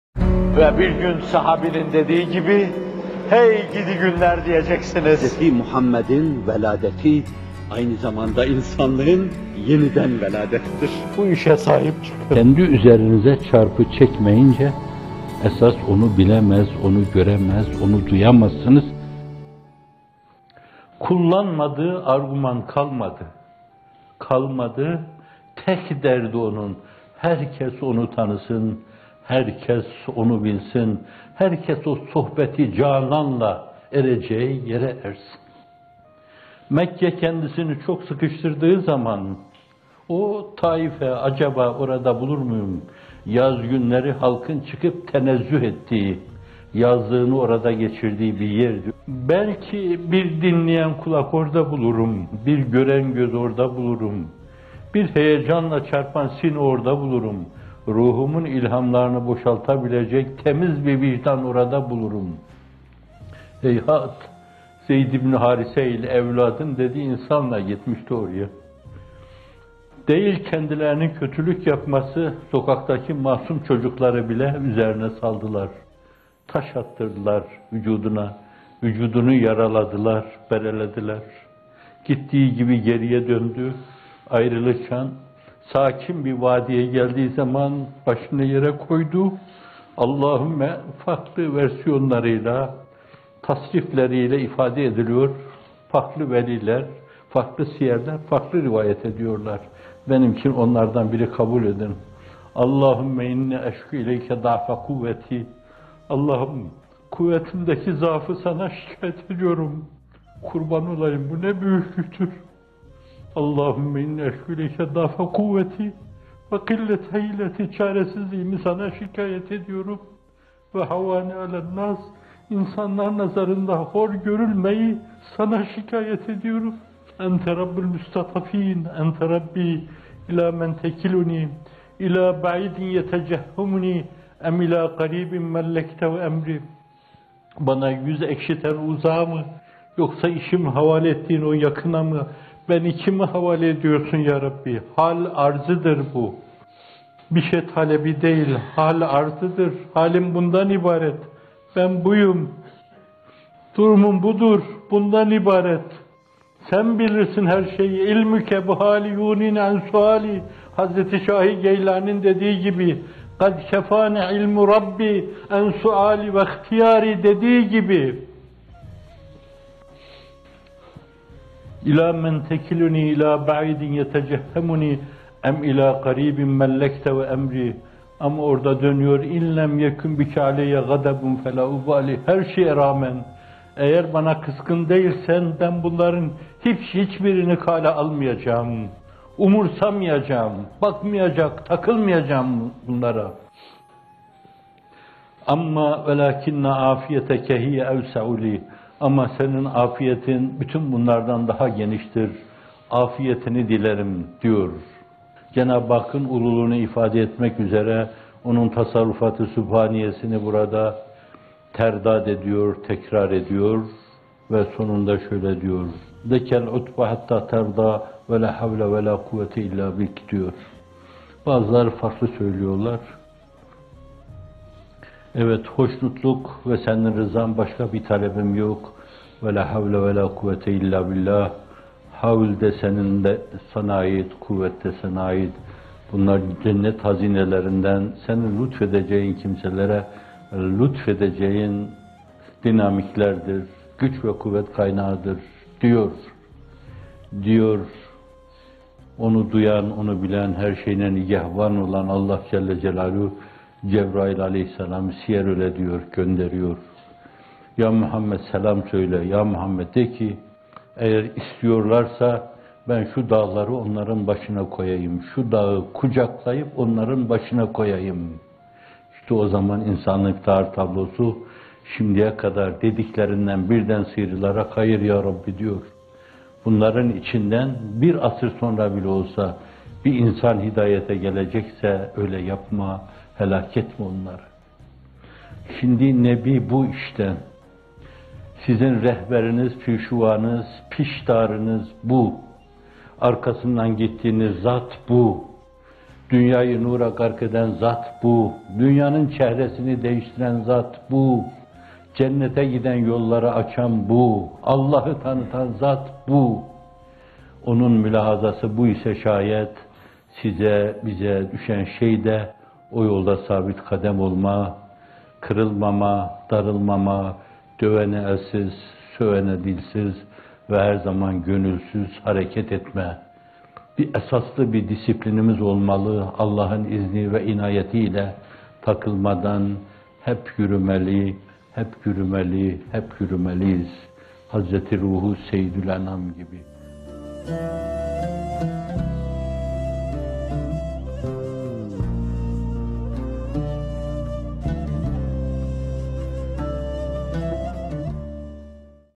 "Hakir Görülmemi Sana Şikâyet Ediyorum" | Mizan | M. Fethullah Gülen Hocaefendi
Bu video 06/03/2016 tarihinde yayınlanan “Off Bile Demediler, Off Bile Demeyeceğiz!..” isimli bamtelinden alınmıştır.  Tamamı burada

“Ya Erhamerrahimîn! Sen hor ve hakir görülen biçarelerin Rabbisin; benim de Rabbimsin.. beni kime bırakıyorsun?!.”

*Peygamber Efendimiz (aleyhi ekmelüttehâyâ) bilhassa Mekke döneminde çok büyük musîbetlerle karşı karşıya kalmıştır; kavmi tarafından yalanlanmış, işkencelere maruz bırakılmış, ölümle tehdit edilmiş ve hatta kendisine komplolar kurulmuştur. Diğer taraftan, kendisinin, ailesinin güzîde fertlerinin ve Ashâb-ı Kirâm’ın esaretten işkenceye, hastalıktan ölüme kadar pek çok imtihanına şahit olmuştur. Fakat Rehber-i Ekmel Efendimiz, hiçbir zaman kaderi tenkit manasına gelebilecek bir şikâyette bulunmamış; belki çok incindiği anlarda Mevlâ-yı Müteâl’e halini arz ederek O’nun rahmetine sığınmıştır. Ezcümle; bir ümitle gittiği Tâif’ten taşlanarak kovulunca, o müsamahasız atmosferden sıyrılıp bir ağacın altına iltica eder etmez, vücudundan akan kana, yarılan başına ve yaralanan ayaklarına aldırmadan Cenâb-ı Hakk’a el açarak söylediği sözler hem pek hazîn hem de kulluk âdâbı adına çok ibretâmizdir:

اَللّٰهُمَّ إلَيْكَ أَشْكُو ضَعْفَ قُوَّتِي وَهَوَانِي عَلَى النَّاسِ، يَا أَرْحَمَ الرَّاحِمِينَ أَنْتَ رَبُّ الْمُسْتَضْعَفِينَ وَأَنْتَ رَبِّي إلَى مَنْ تَكِلُنِي؟ إلَى بَعِيدٍ يَتَجَهَّمُنِي أَمْ إلَى عَدُوٍّ مَلَّكْتَهُ أَمْرِي. إِنْ لَمْ يَكُنْ بِكَ غَضَبٌ عَلَيَّ فَلاَ أُبَالِي، وَلَكِنْ عَافِيَتُكَ هِيَ أَوْسَعُ لِي. أَعُوذُ بِنُورِ وَجْهِكَ الَّذِي أَشْرَقَتْ لَهُ الظُّلُمَاتُ وَصَلَحَ عَلَيْهِ أَمْرُ الدُّنْيَا وَاْلآخِرَةِ مِنْ أَنْ تُنْـزِلَ بِي غَضَبَكَ أَوْ يُحِلَّ عَلَيَّ سَخَطُكَ. لَكَ الْعُتْبَى حَتَّى تَرْضَى وَلاَ حَوْلَ وَلاَ قُوَّةَ إِلاَّ بِكَ

“Allahım, güçsüzlüğümü, zaafımı ve insanlar nazarında hakir görülmemi Sana şikâyet ediyorum. Ya Erhamerrahimîn! Sen hor ve hakir görülen biçarelerin Rabbisin; benim de Rabbimsin.. beni kime bırakıyorsun?!. Kötü sözlü, kötü yüzlü, uzak kimselere mi; yoksa, işime müdahil düşmana mı? Eğer bana karşı gazabın yoksa, Sen benden razıysan, çektiğim belâ ve mihnetlere hiç aldırmam. Üzerime çöken bu musîbet ve eziyet, şayet Senin gazabından ileri gelmiyorsa, buna gönülden tahammül ederim. Ancak afiyetin arzu edilecek şekilde daha ferah-feza ve daha geniştir. İlâhî, gazabına giriftâr yahut hoşnutsuzluğuna düçâr olmaktan, Senin o zulmetleri parıl parıl parlatan dünya ve ahiret işlerinin medâr-ı salâhı Nûr-u Vechine sığınırım; Sen razı olasıya kadar affını muntazırım! İlâhî, bütün havl ve kuvvet sadece Sen’dedir.”

*Hâşâ, biz Nebiler Serveri’nin kendi muhasebesini yaparken dile getirdiği bu ifadeleri lazımî manasıyla ele alamayız; bir yönüyle, O’nun kendi hakkındaki sözlerini zikrederken dahi su-i edepte bulunmuş sayılırız. Fakat O’nun tevazu, mahviyet ve kulluk edebine riayet gibi hasletlerini hesaba katarak meseleye baktığımızda, nefsini yerden yere vurduğunu, meseleyi -hâşâ ve kellâ- kendi yetersizliğine bağladığını ve Cenâb-ı Hakk’ın inayetine, vekâletine, kilâetine sığındığını görürüz.

*Rasûl-ü Ekrem Efendimiz, Tâif dönüşü o hazin münacatı karşısında Hazreti Cebrail’in gelip “Ya Muhammed (sallallâhu aleyhi ve sellem), istersen dağlar meleği şu dağı Taiflilerin başına geçirsin!” demesi karşısında adeta tir tir titremiş; “Hayır! Bu insanların neslinden yüz yıl sonra bile bir mü’min gelecekse, böyle bir şeyin olmasını istemem!” diye inlemişti.

*Şimdi, Nebi bu işte!.. Sizin rehberiniz, pîşuvânız, pişdârınız bu!.. Arkasından gittiğiniz Zât bu!.. Dünyayı nura gark eden zat bu!.. Cennet’e giden yolları açan bu, Allah’ı tanıtan Zât bu!.. O’nun mülahazası buysa şayet, size, bize düşen vazife de o yolda sabit kadem olmaktır.

►Bizimle paylaşmak istediğiniz haberler için e-posta adresimiz 👉